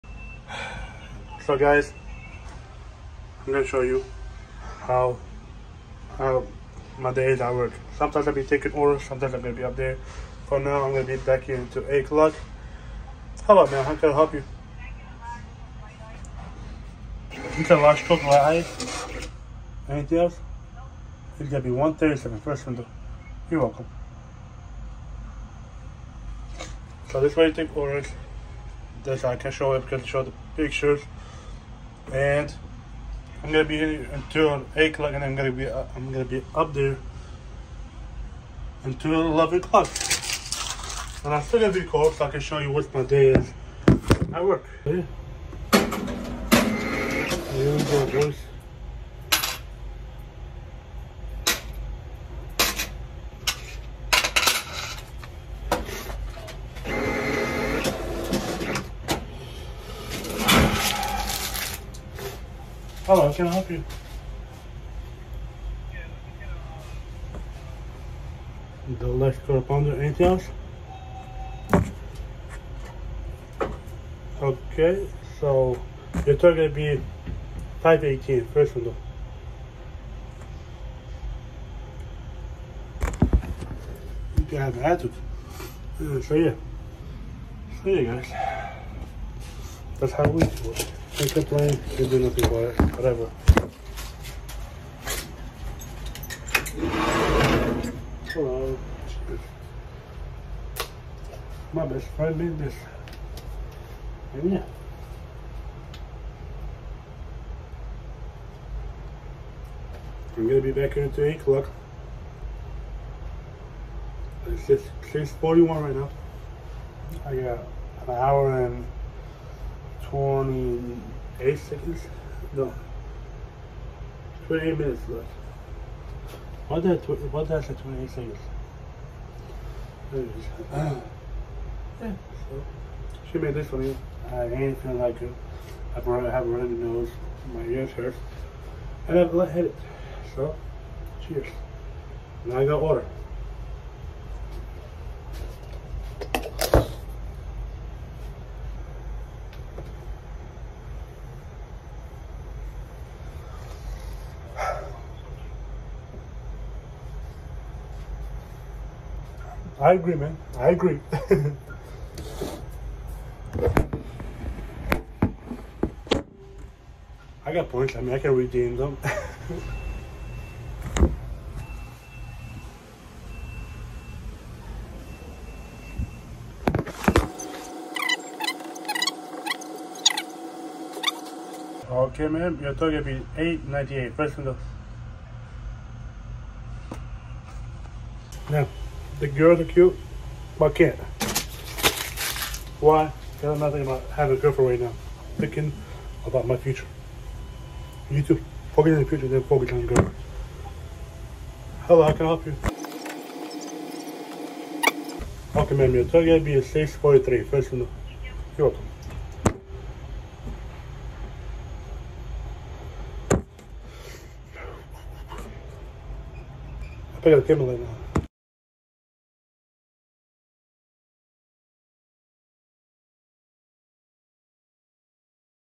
A Day In The Life as a McDonald's Employee…Fast Food Vlog
This is a part one of in a day working in a McDonald's or as a McDonald's employee also enjoy you enjoy make sure to like subscribe and turn on the notification bell.

Shutout to McDonald's

Link to my TikTok page